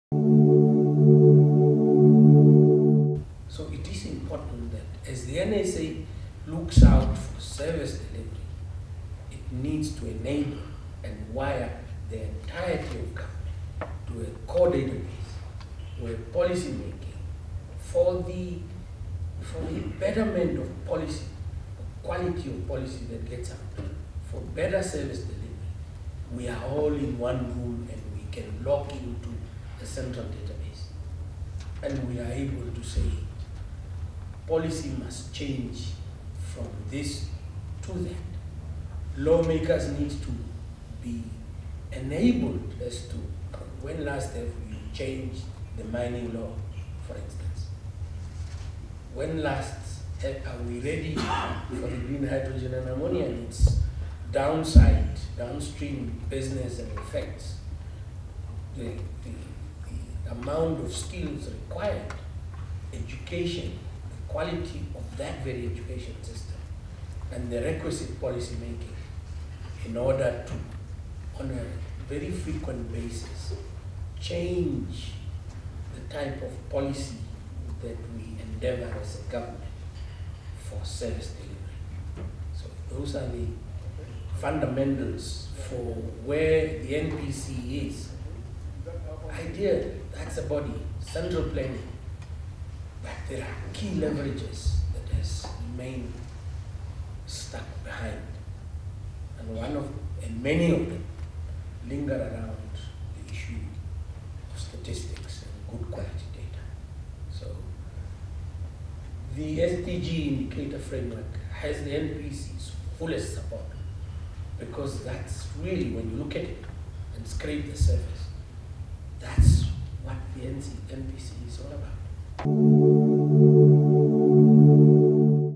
WATCH: Kandjoze - Cabinet makes decisions on outdated data
Cabinet makes decisions on outdated data read the full story in today's New Era